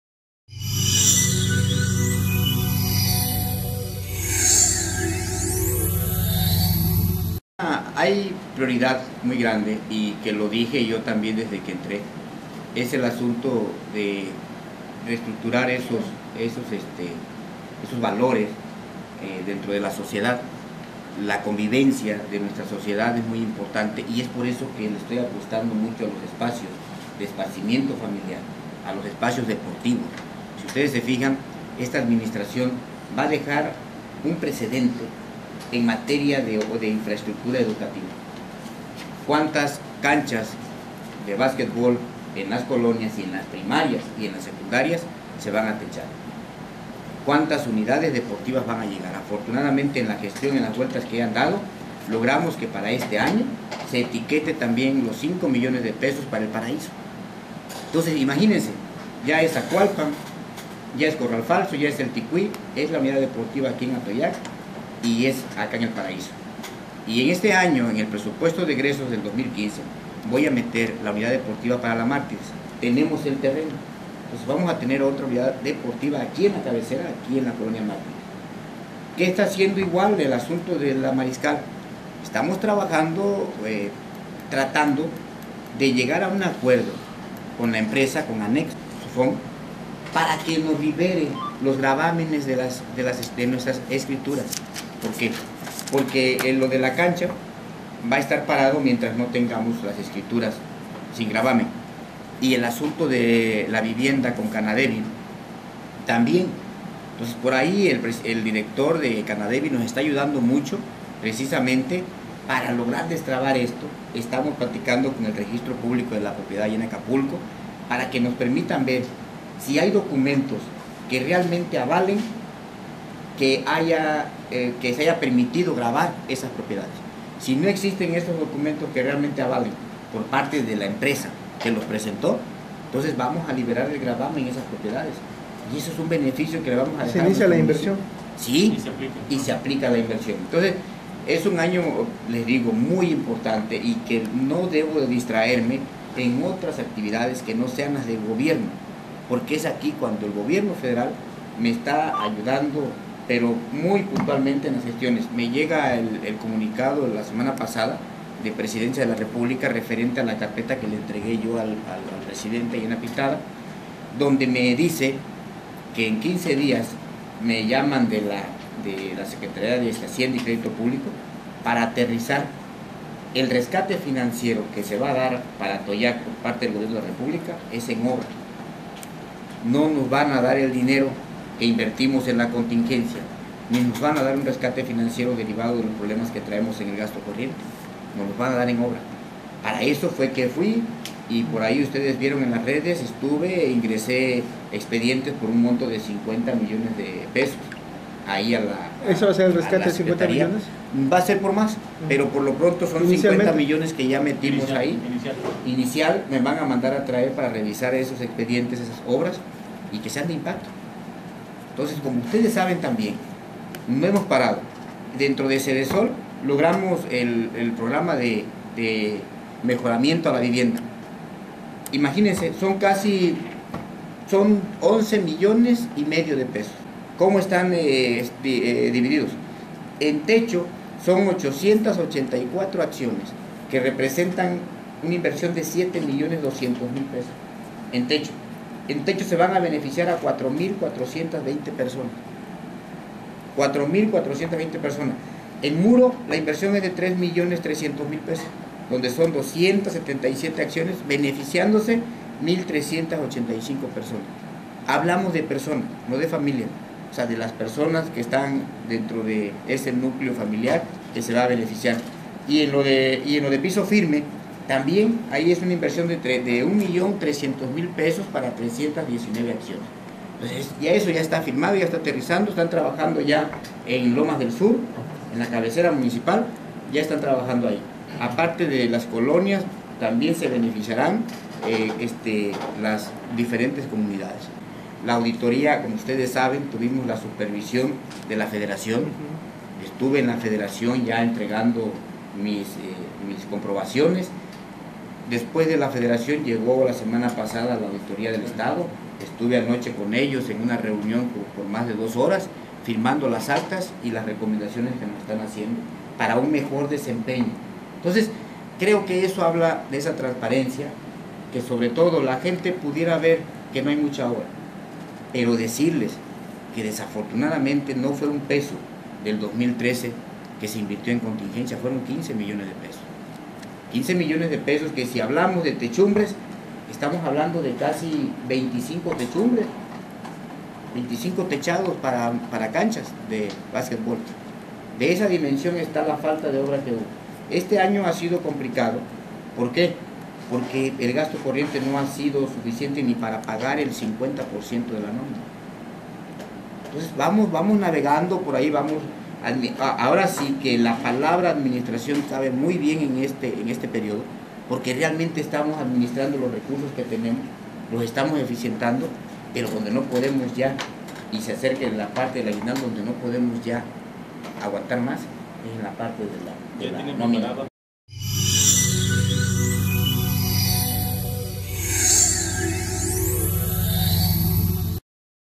MAS INVERSION A SOCIALIZAR A LOS ATOYAQUENSES: TABAREZ CISNEROS
El gobierno de Atoyac se ha venido administrando; debido a los errores y omisiones del pasado, que afecta el presente: Tabarez Cisneros
• Importante inversión para más de 25 techumbres de canchas deportivas.
• Estamos aprovechando los espacios, para socializar a las familias.